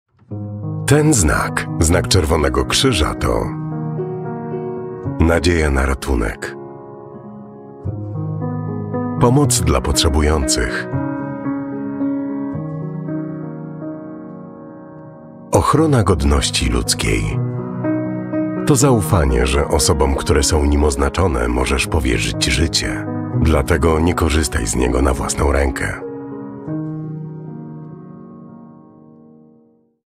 The Red Cross emblem (ENG)
The Red Cross emblem is a mark protected by international law and the laws of the Republic of Poland. They define in detail who can use it and when, so that it fulfils its purpose – to mark the help for those in need and to protect those who help.